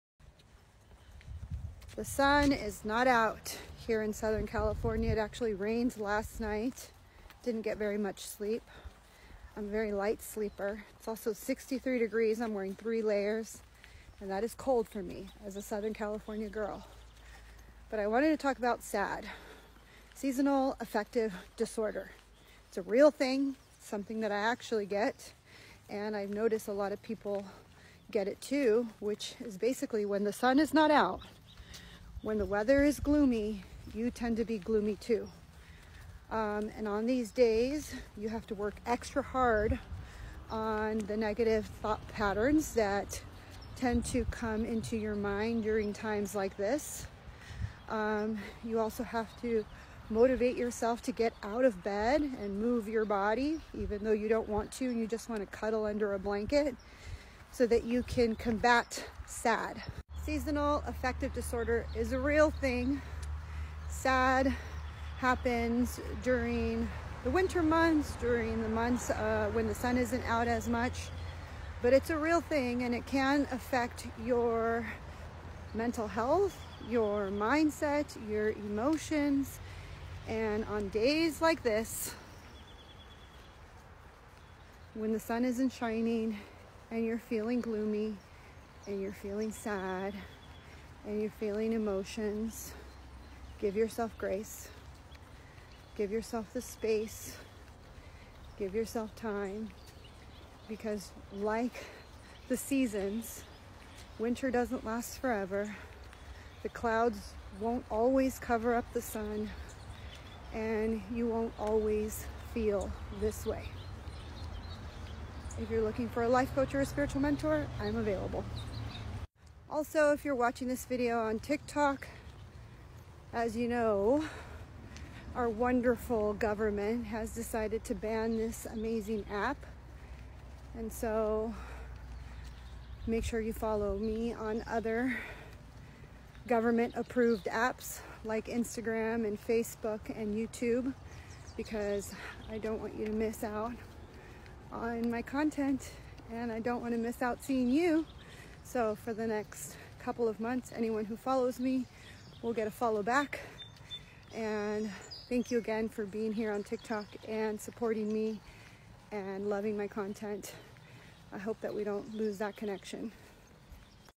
What is Seasonal Affective Disorder? The Truth Behind SAD Syndrome
Discover the truth behind Seasonal Affective Disorder (SAD) in this informative video. Learn about the symptoms, causes, and treatment options for SAD syndrome, a type of depression that occurs seasonally. Find out how to recognize the signs and what you can do to manage and cope with this common mental health condition. Watch now to gain a better understanding of SAD and how it may be affecting you or your loved ones.